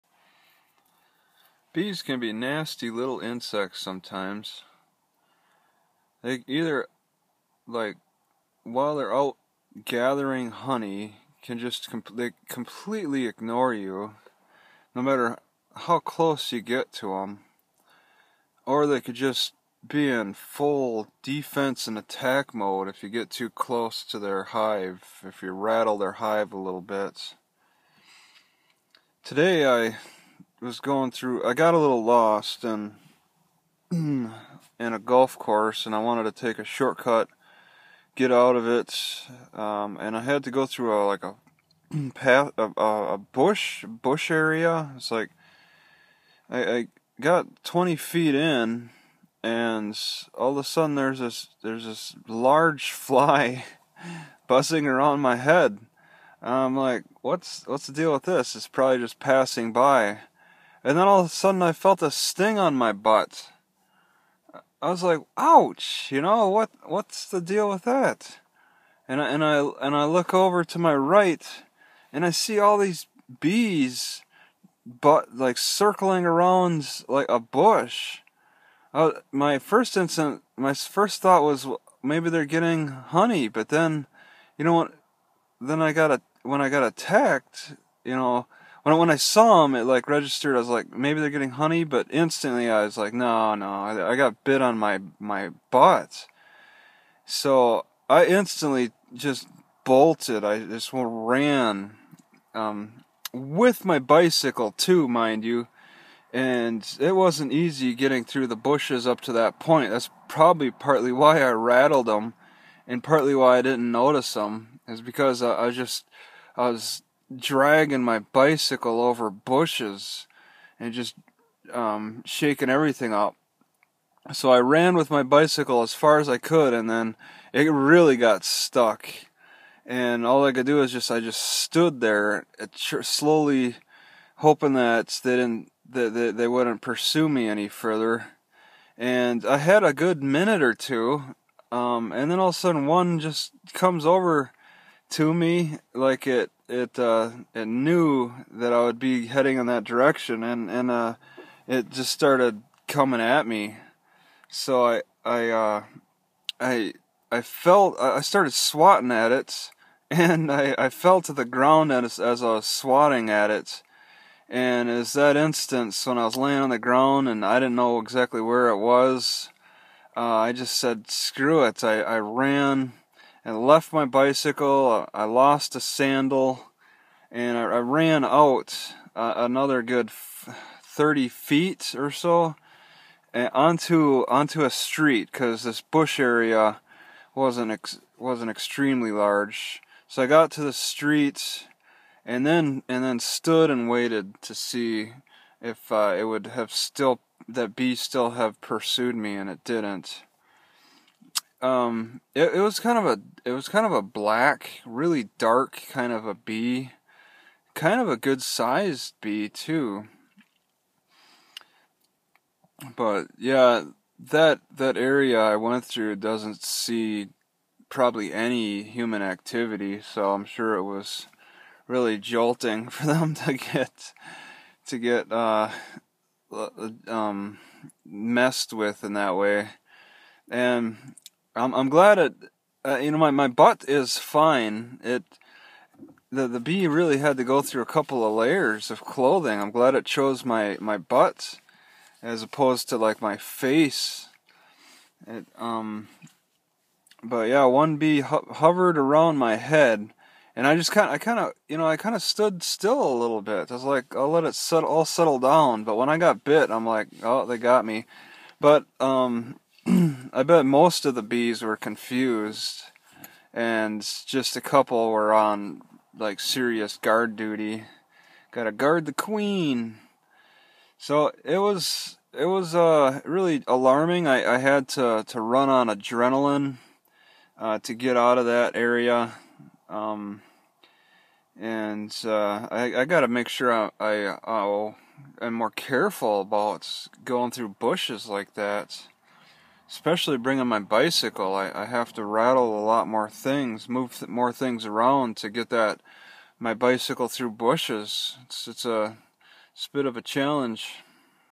Vlog: A Bee Stung my Butt Today, but caused no permanent damage, It was my fault.
I have been walking through a lot of bushes, but never came across a bee hive in them before. This was a surprise.